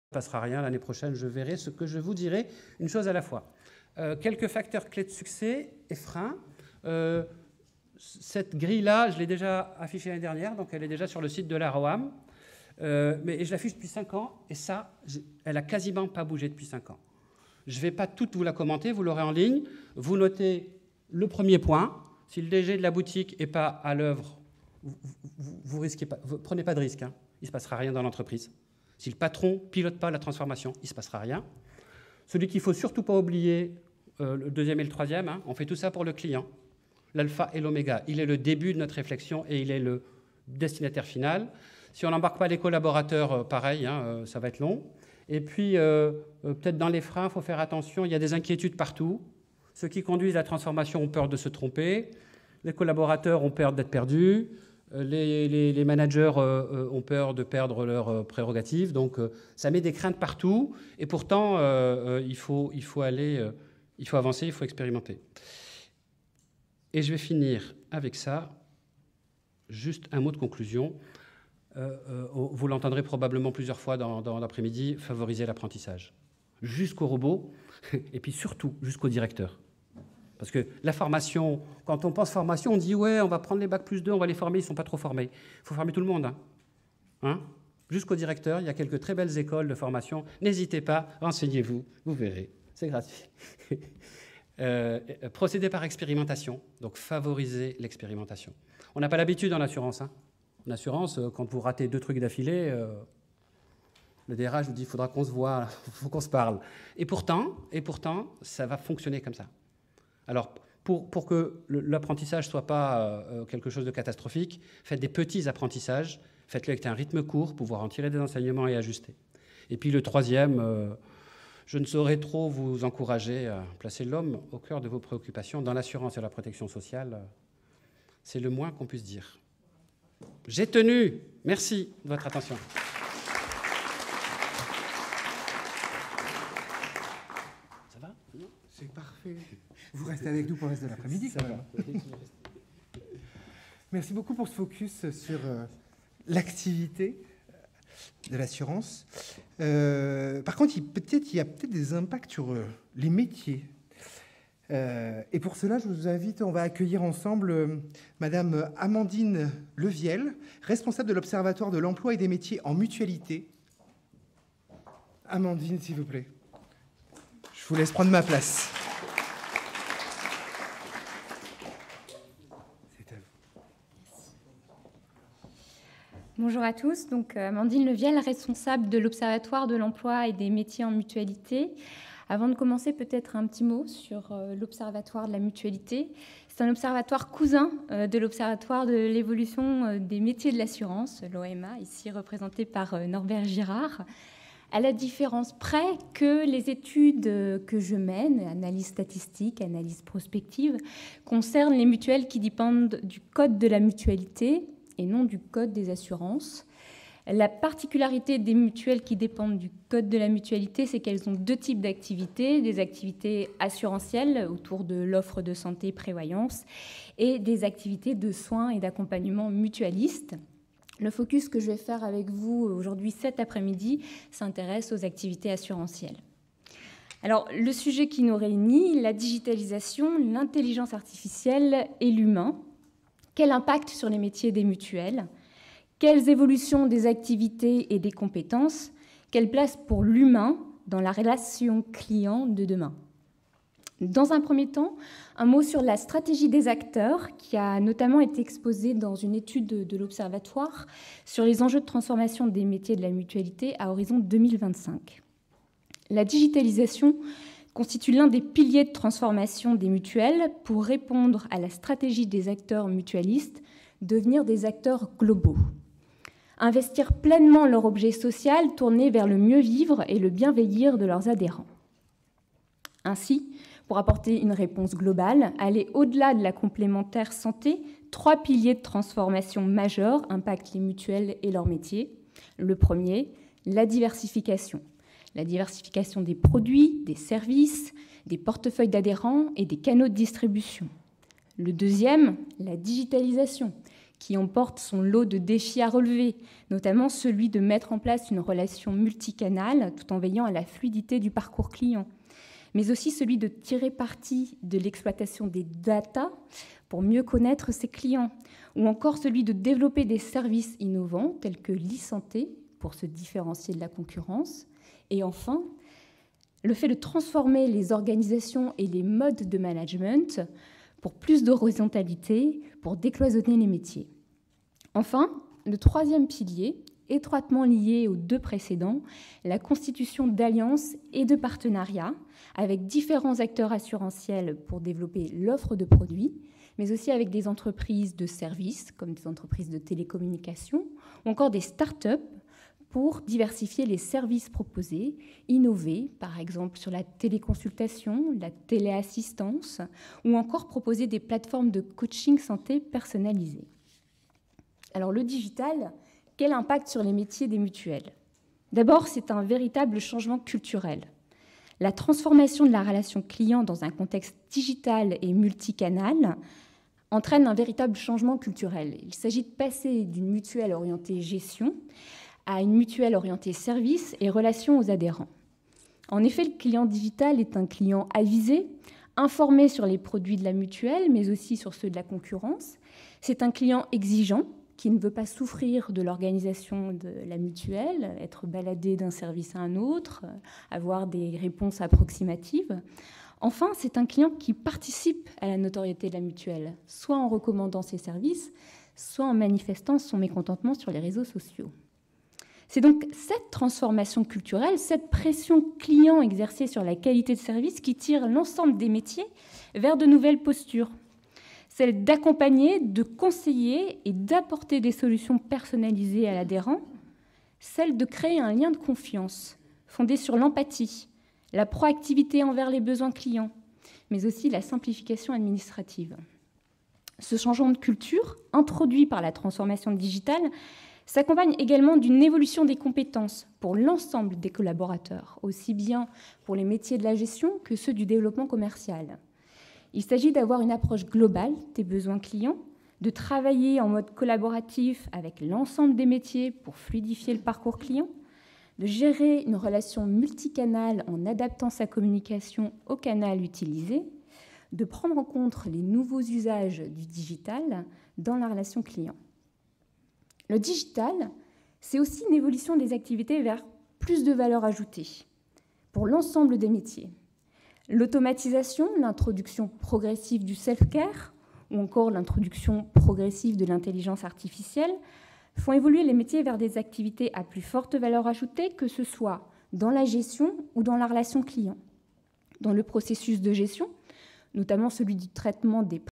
Intervention d'Amandine LEVIEL - Conf. des Experts Roam 2018
Intervention d'Amdnine Leviel à la 2ème édition des "Conférences des Experts" Roam du 6 juin 2018 - Auditorium de la Fédération Française de l'Assurance (FFA)